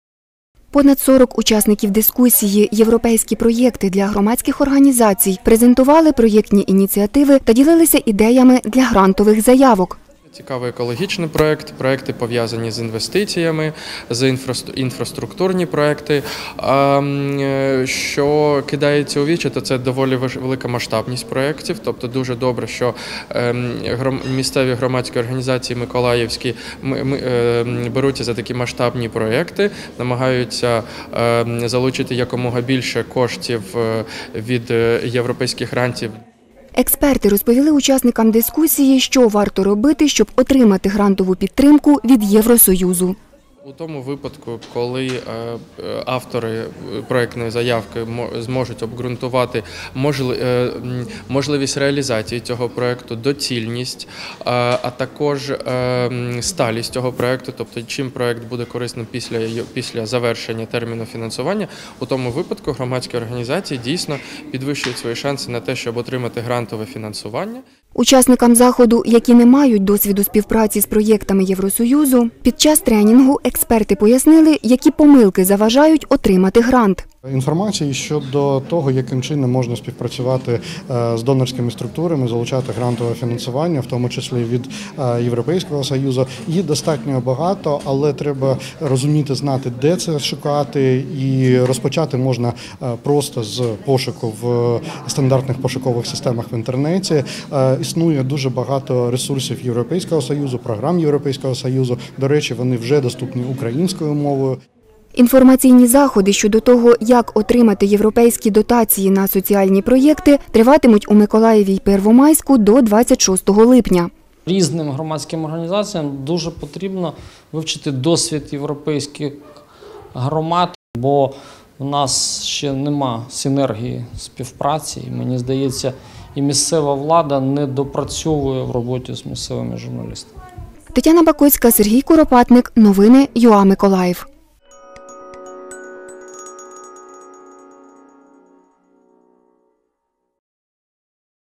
Триває дискусія «Європейські проєкти для громадських організацій»
У Миколаєві провели дискусію-тренінг  «Європейські проєкти для громадських організацій». Понад сорок учасників дискусії  презентували проєктні ініціативи та ділилися ідеями для грантових заявок.
Авторка – Т. Бакоцька
Оператор – С. Куропятник